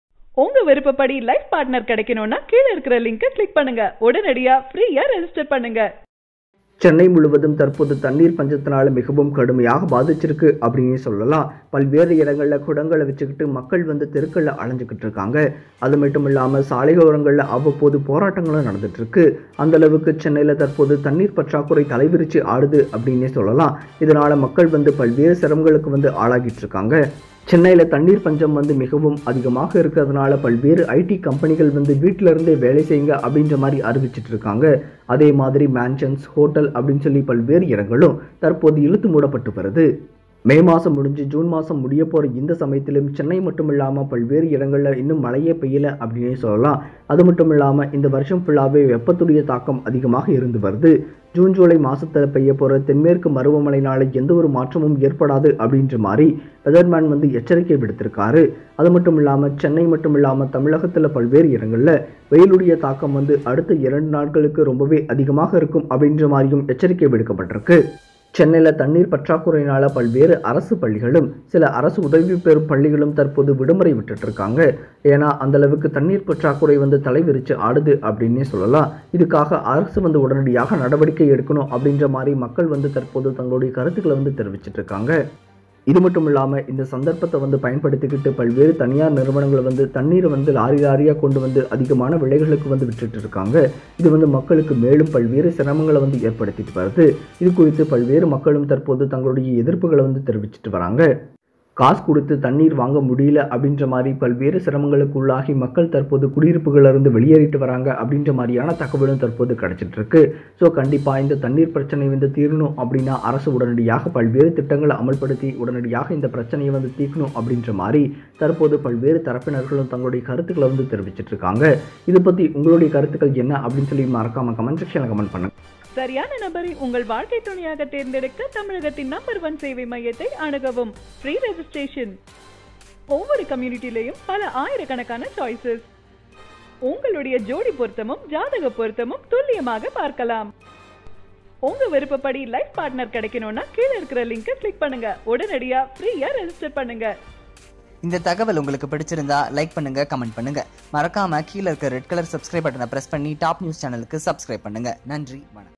சென்னையில் தண்ணீர் பிரச்சனை | பள்ளிகளுக்கு விடுமுறை | Tamil News
Referal Code : 066YB7

சென்னையில் தண்ணீர் பிரச்சனை | பள்ளிகளுக்கு விடுமுறை | அதிர்ச்சி வீடியோ | Tamil News

For all the latest updates on Kollywood movies, celebrities   Tamil News, Latest Tamil News